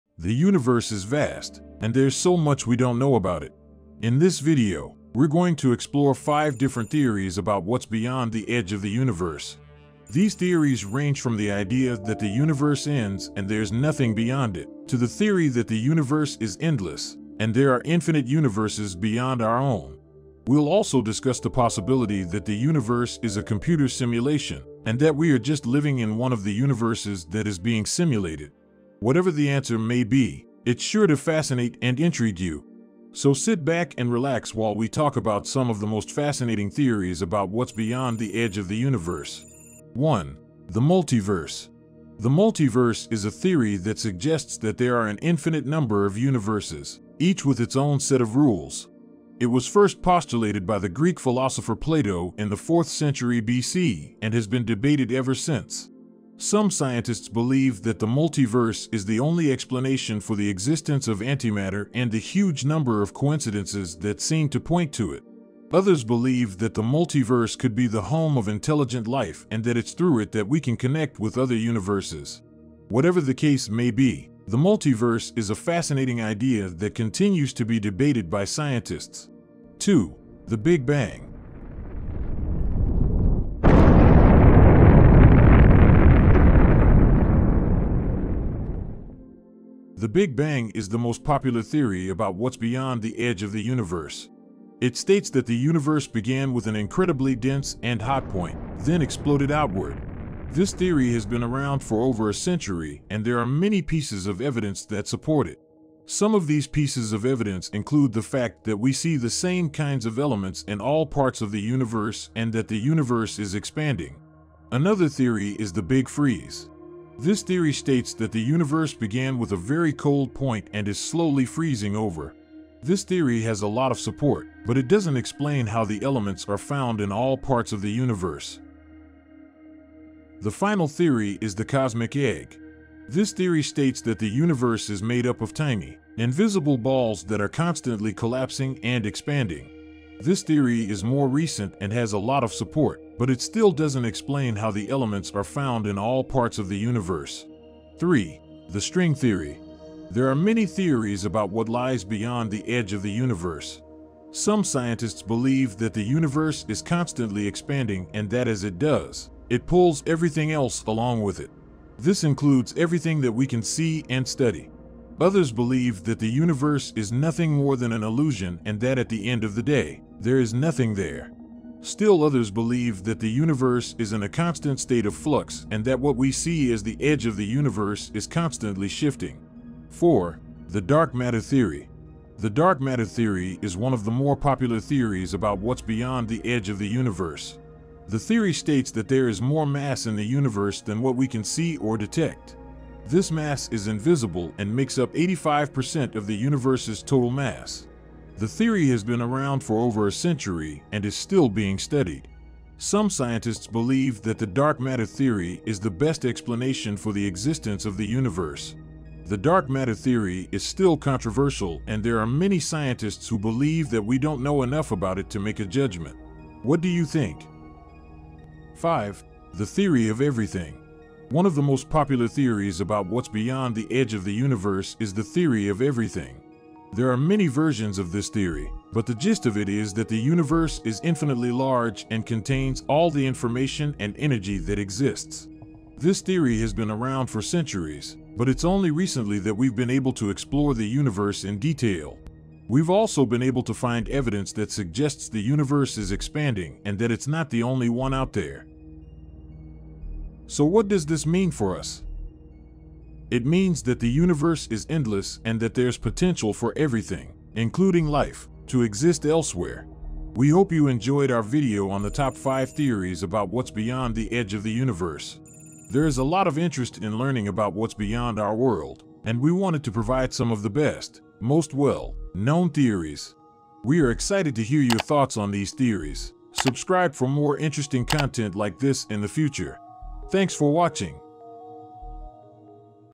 What's Beyond The Edge of the Universe?
The universe is vast, and there's so much we don't know about it. In this video, we're going to explore five different theories about what's beyond the edge of the universe. These theories range from the idea that the universe ends and there's nothing beyond it, to the theory that the universe is endless, and there are infinite universes beyond our own. We'll also discuss the possibility that the universe is a computer simulation, and that we are just living in one of the universes that are being simulated. Whatever the answer may be, it's sure to fascinate and intrigue you. So sit back and relax while we talk about some of the most fascinating theories about what's beyond the edge of the universe. We hope you enjoyed our video on the top 5 theories about what's beyond the edge of the universe. There is a lot of interest in learning about what's beyond our world, and we wanted to provide some of the best, most well-known theories.

*Here is an article listing some crazy interesting and amazing facts about the universe and what could lie beyond the edge*